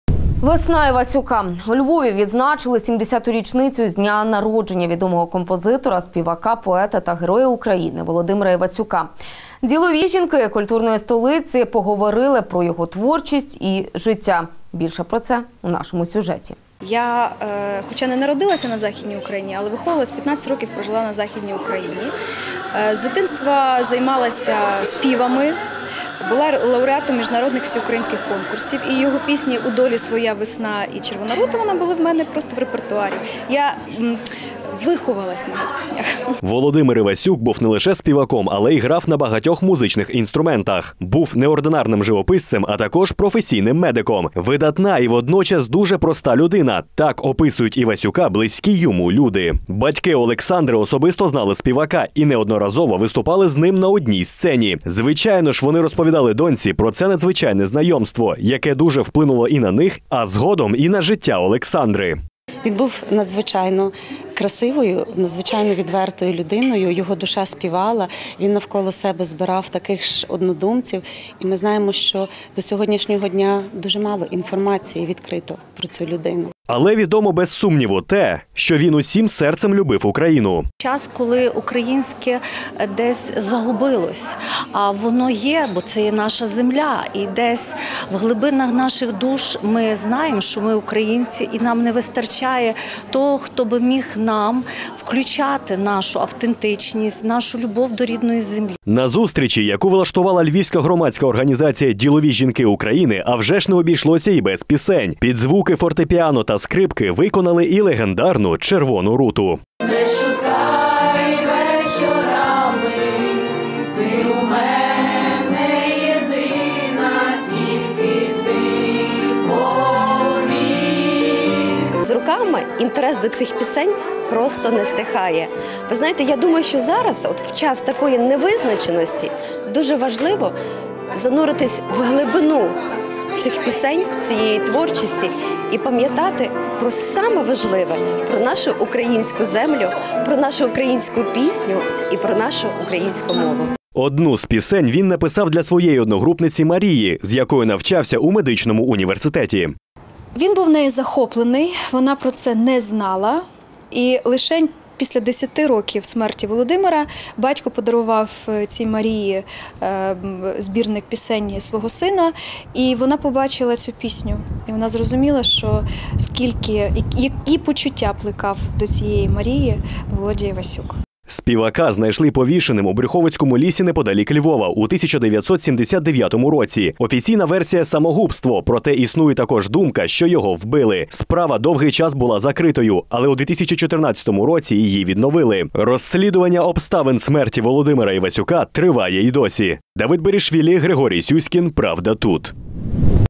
Весна Івасюка. ПравдаТУТ Львів
Весна Івасюка. У Львові відзначили 70-ту річницю з дня народження відомого композитора, співака, поета та Героя України Володимира Івасюка. "Ділові жінки" культурної столиці поговорили про його творчість і життя.